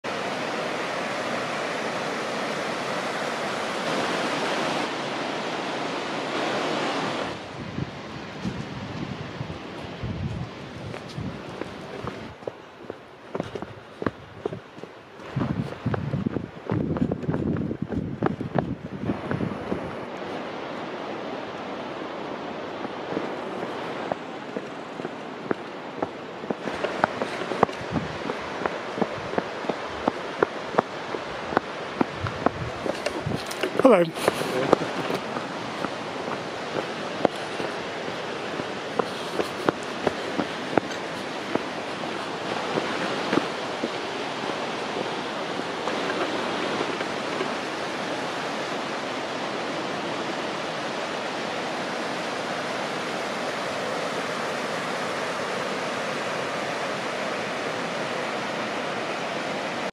Hike From Tower Falls To The Yellowstone River! Part 1
There is a steep, but pretty, short hike from the Tower Falls overlook down to the Yellowstone River. The steepness, unfortunately, is on the way back!